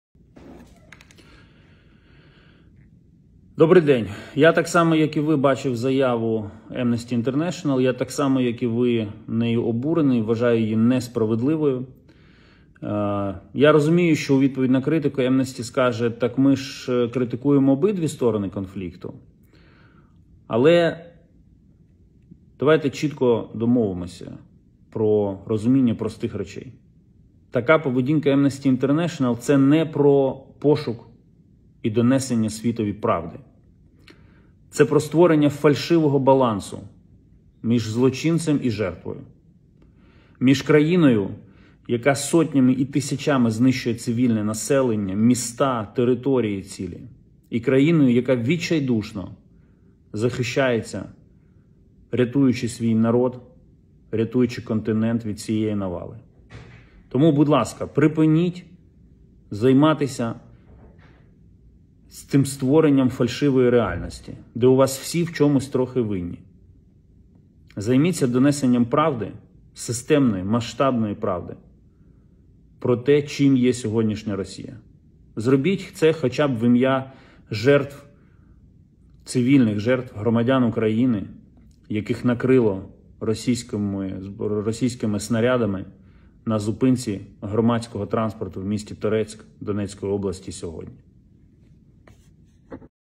Кулеба - Amnesty International: Не шукайте фальшивого балансу між злочинцем і жертвою
Міністр закордонних справ Дмитро Кулеба закликав правозахисну організацію Amnesty International не шукати фальшивого балансу між країною-злочинцем росією і Україною, яка захищається від неї, а розповісти світові правду про росію.

Про це глава української дипломатії заявив у Фейсбуці, передає Укрінформ.

Глава МЗС зазначив, що його, як і багатьох українців, обурила заява Amnesty International. Він також припустив, що у відповідь на критику Amnesty International скаже, що критикує обидві сторони конфлікту.

«Але давайте чітко домовимося про розуміння простих речей: така поведінка Amnesty International – це не про пошук і донесення світові правди, це про створення фальшивого балансу між злочинцем і жертвою, між країною, яка сотнями і тисячами знищує цивільне населення, міста, цілі території, і країною, яка відчайдушно захищається, рятуючи свій народ і континент від цієї навали», - сказав Кулеба.

Звертаючись до Amnesty International , він закликав правозахисну організацію "припинити займатися створенням фальшивої реальності, де у вас усі в чомусь трохи винні».

Відео: фейсбук-сторінка міністра закордонних справ Дмитра Кулеби